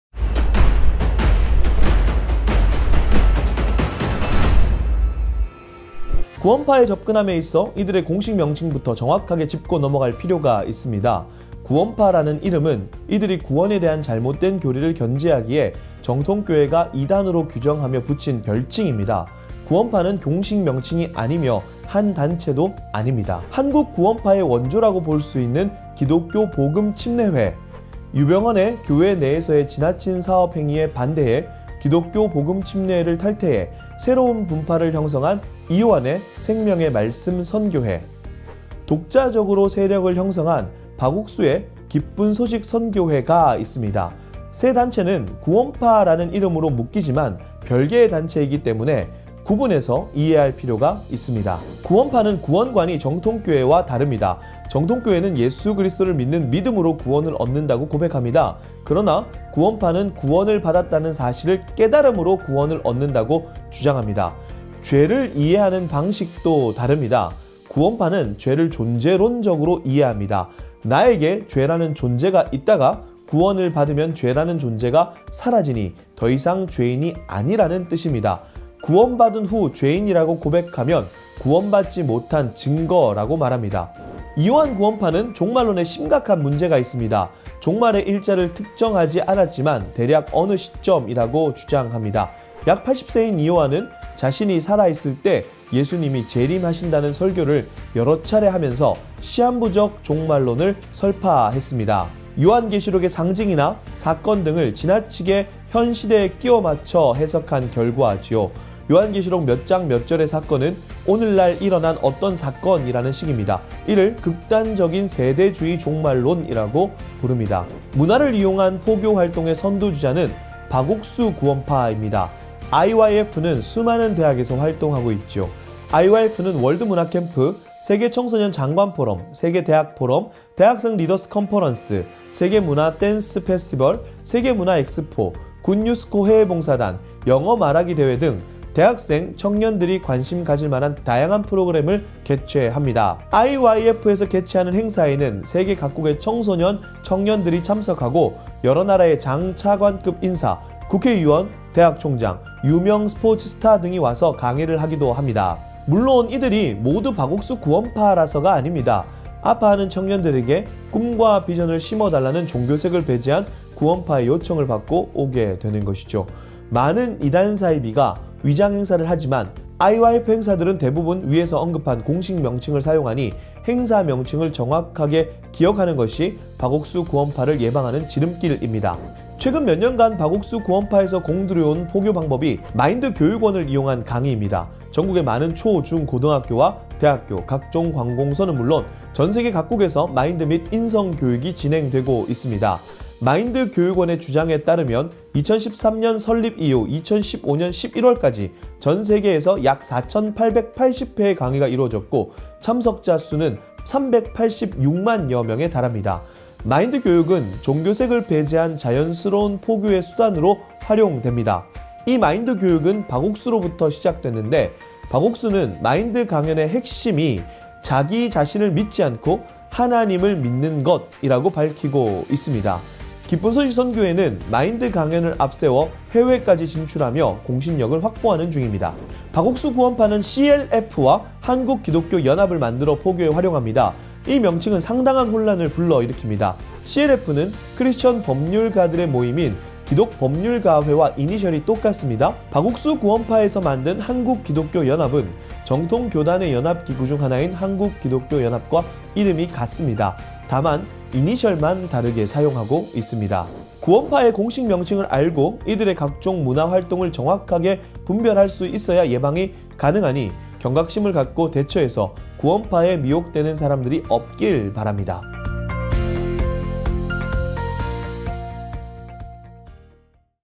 구원파
바른미디어가 주요 이단 사이비 8개 단체의 간략한 정보를 제공하는 영상을 제작했다.